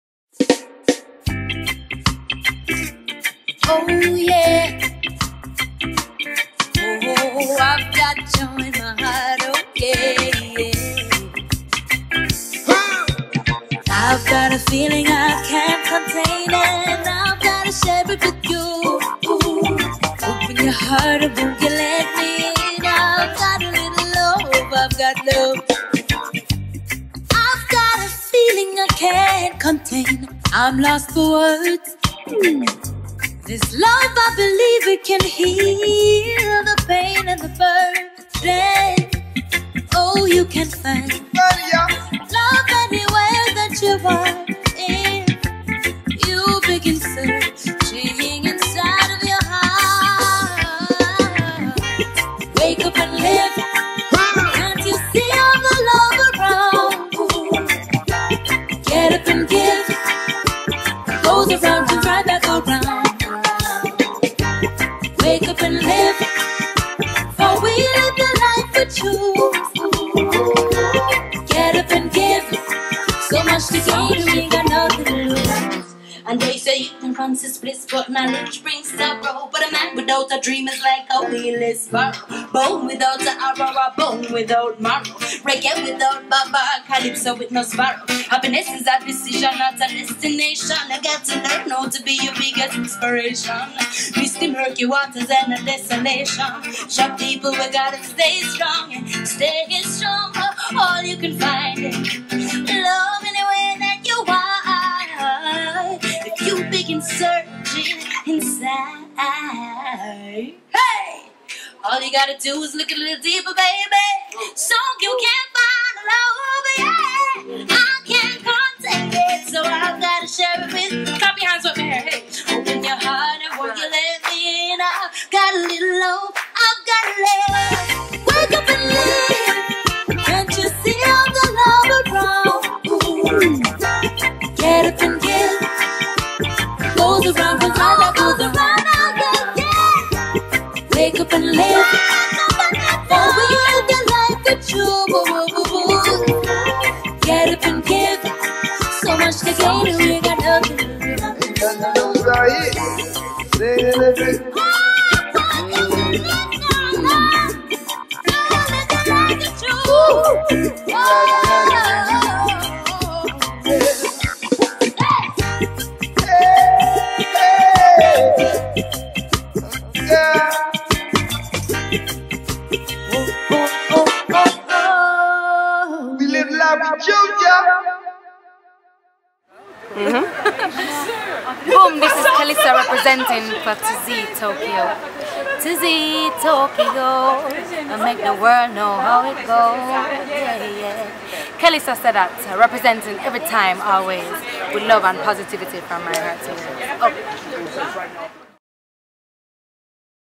KELISSA SESSION AT ITAL KITCHEN
Official T.O.K.Y.O JOINT PRESENTS:

Kelissa LIVE acoustic Session at ital Kitchen
GOOD FOOD
GOOD PEOPLE
GOOD VIBEZ
heres an inside look into Ital Kitchen as they prepare a delicious vegan meal as the people enjoy musical selections by Kelissa